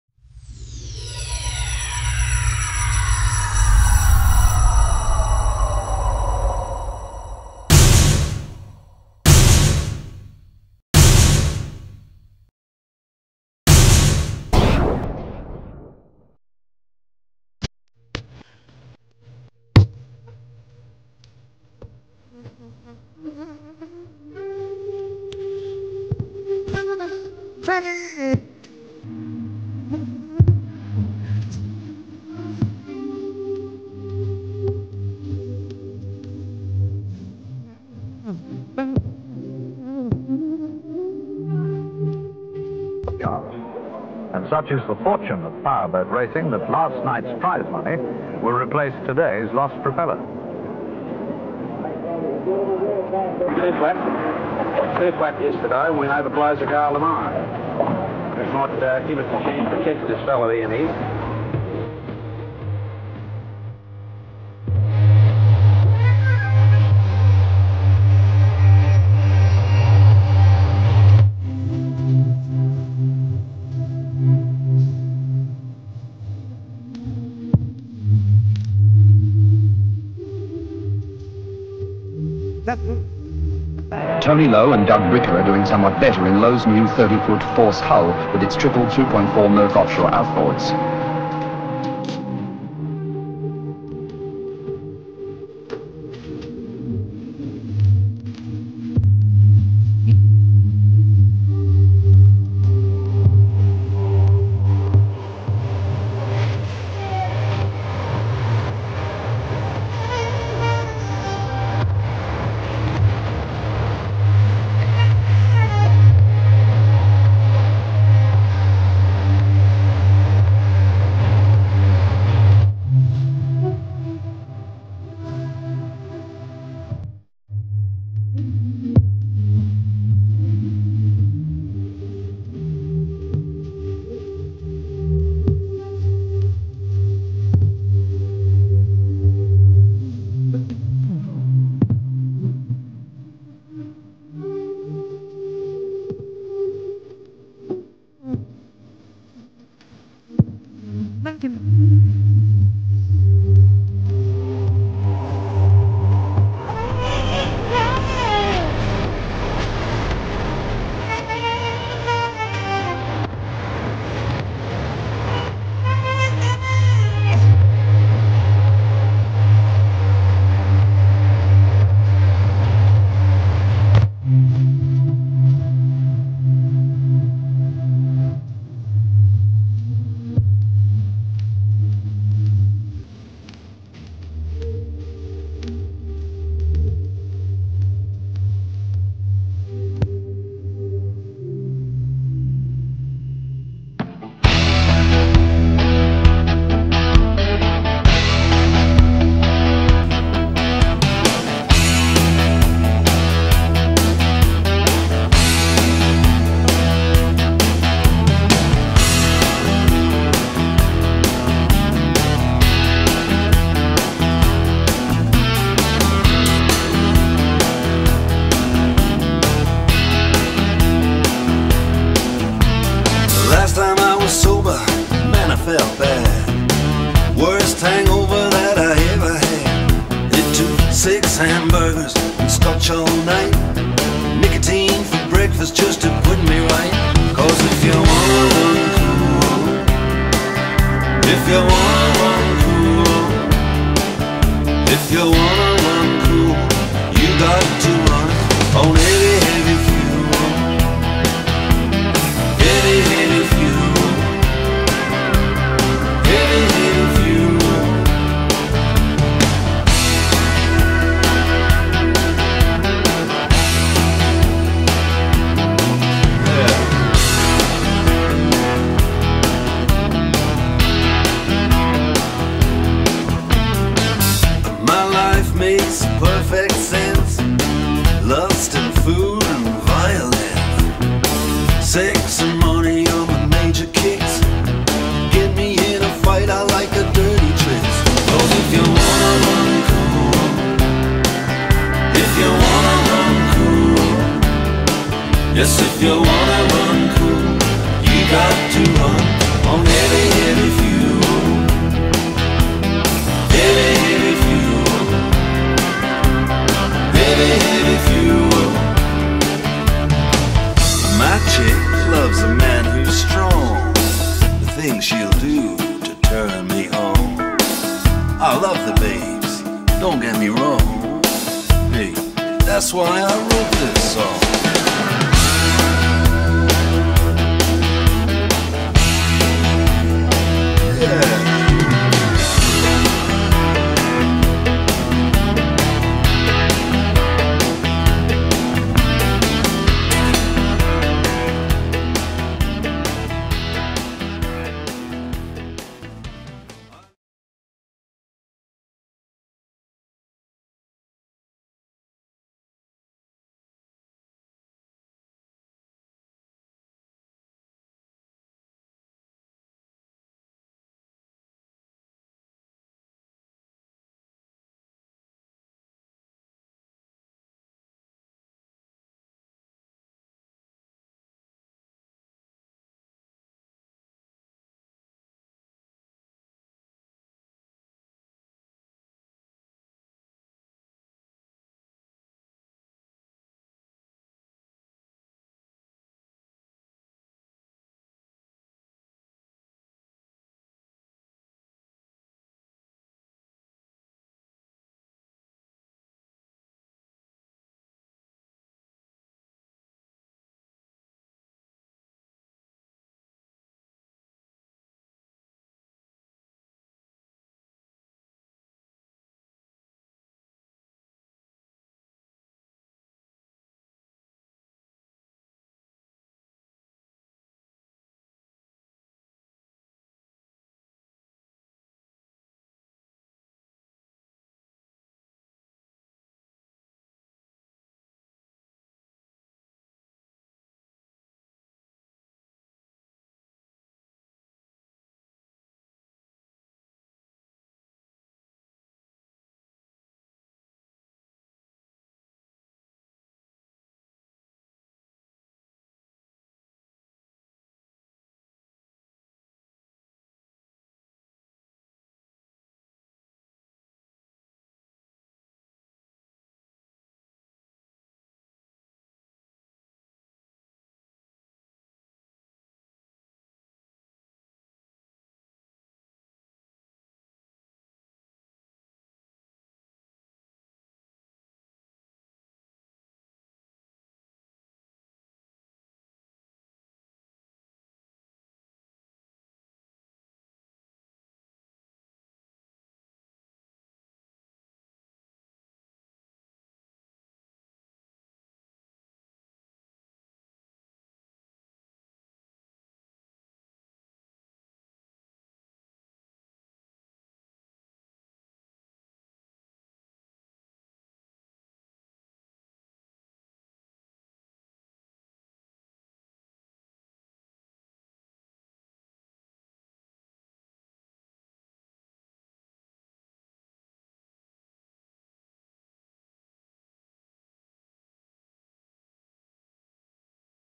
TONY LOW - APBA HALL OF FAME VIDEO
Tony Low - Powerboat racer, boat builder, clubman, legend!